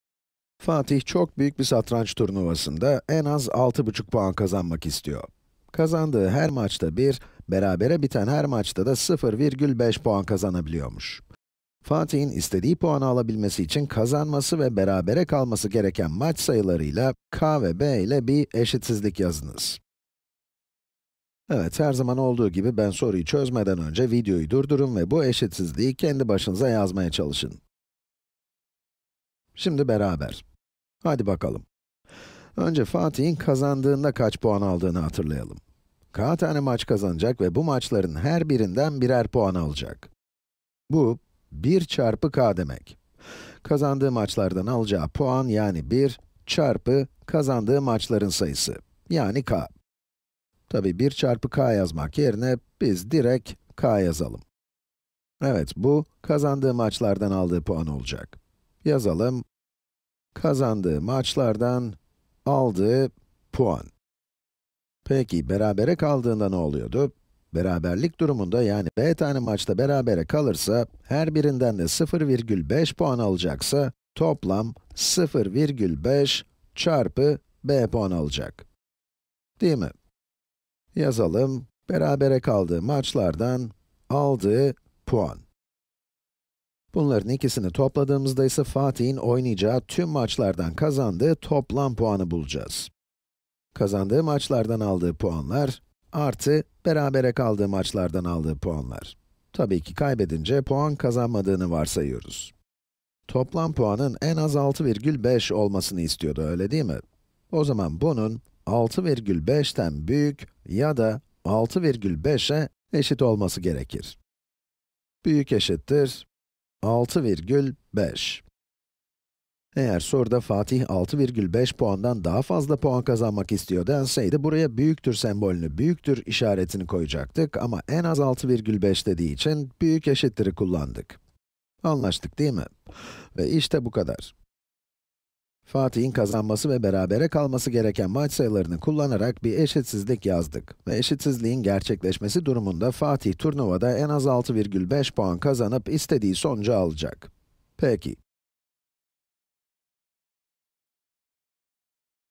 İki Değişkenli Eşitsizlik Yazma Problemleri (Cebir)
Writing two-variable inequalities word problem